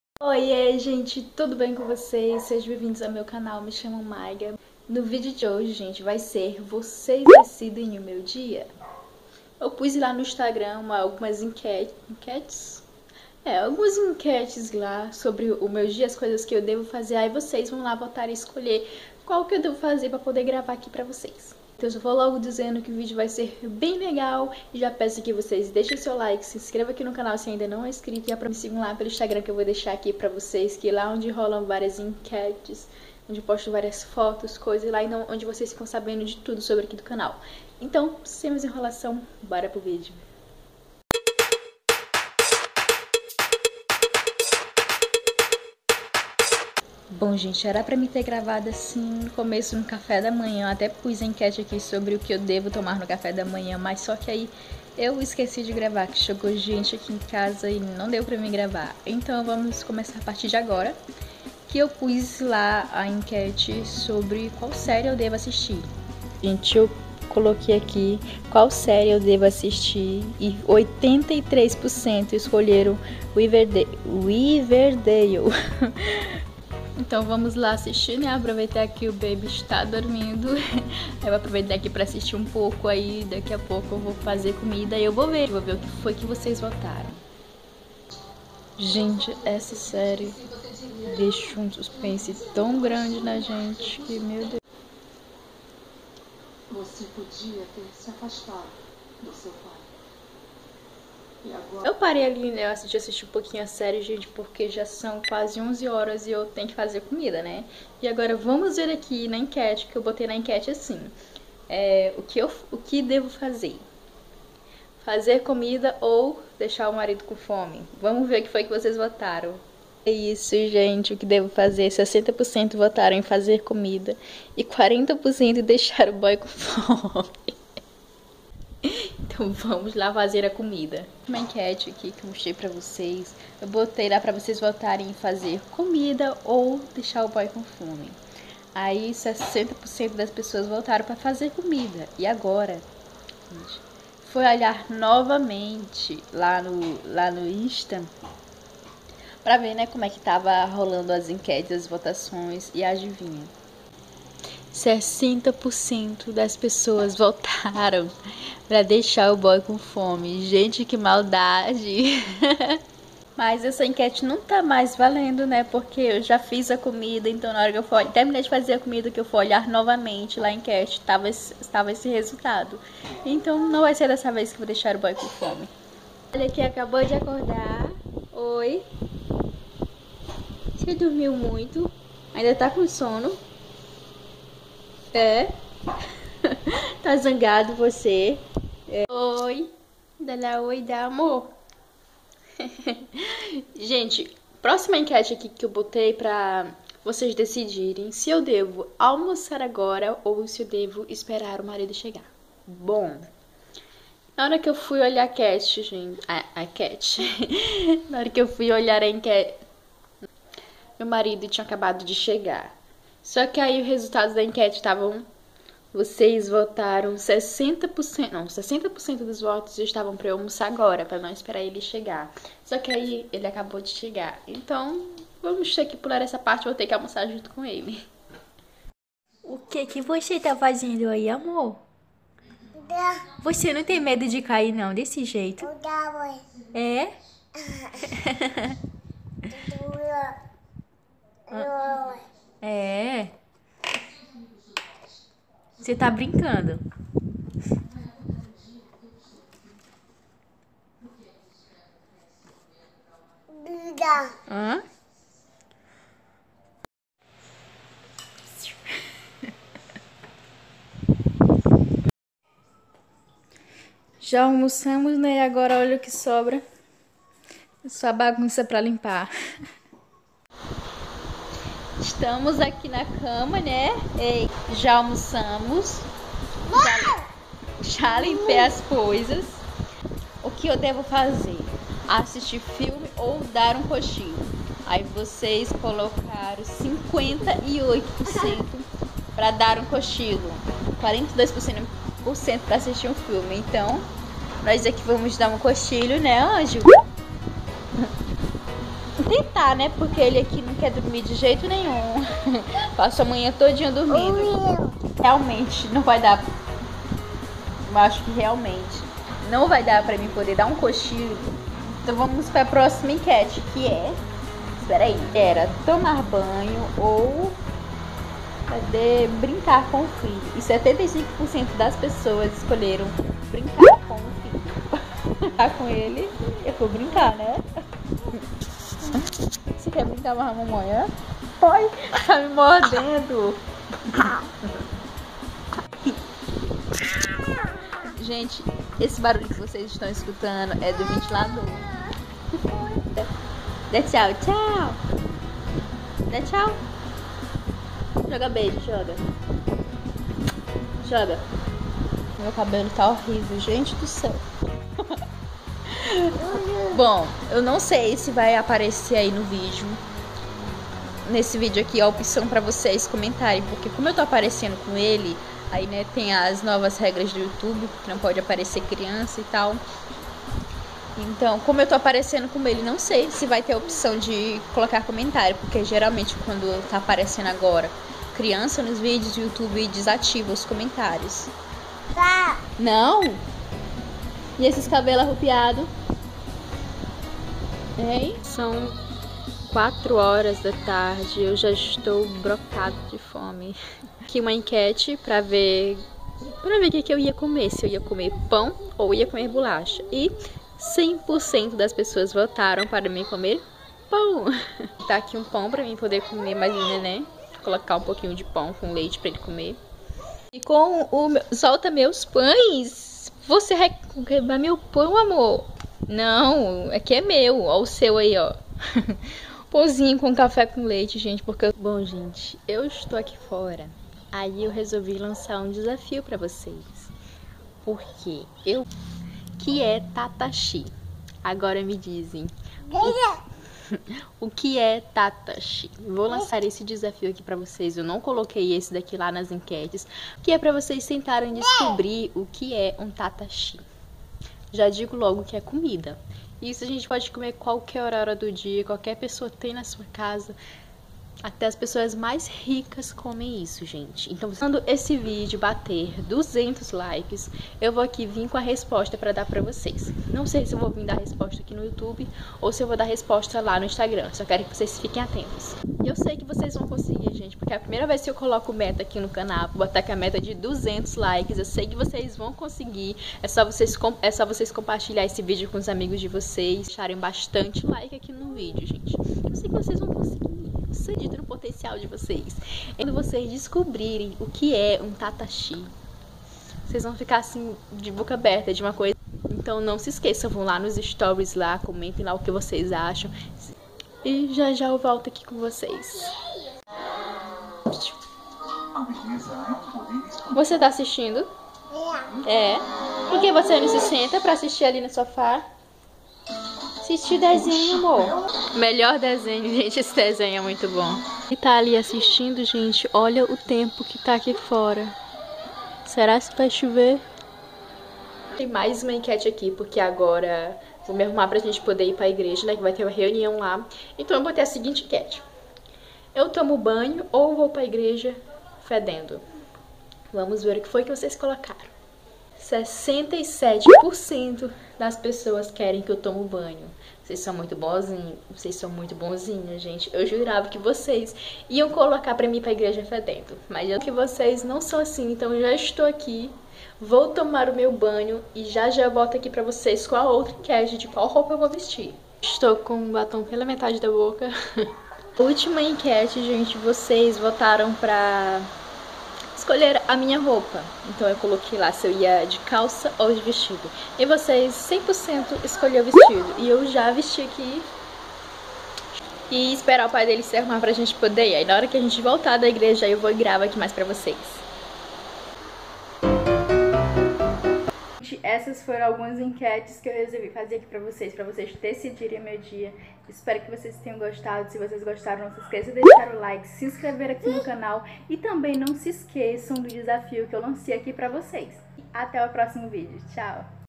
VOCÊS DECIDIRAM O MEU DIA | ENQUETES DO INSTAGRAM
✔NÃO SE ESQUEÇA DE DEIXAR NOS COMENTÁRIOS OU NOS STORY DO INSTAGRAN O QUE VOCÊ ACHA QUE É "TATACHI"❤

   💕 Oie gente tudo bem com vocês?
O vídeo de hoje vocês decidiram o meu dia através do Instagram. ..Espero que gostem!

🔷 Compartilhe